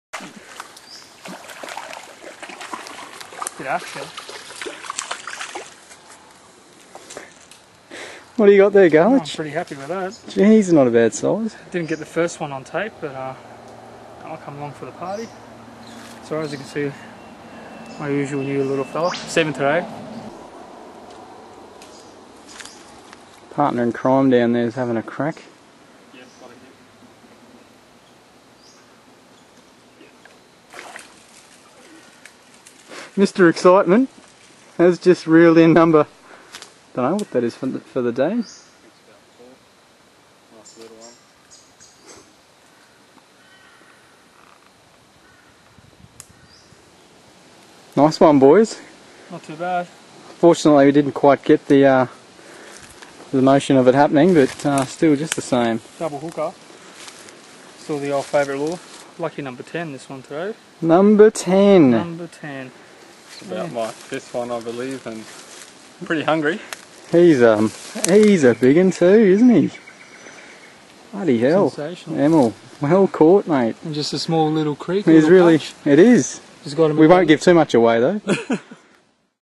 Smashing some Aussie Bass
Croat Brothers catch some Aussie Bass on lures in a local creek.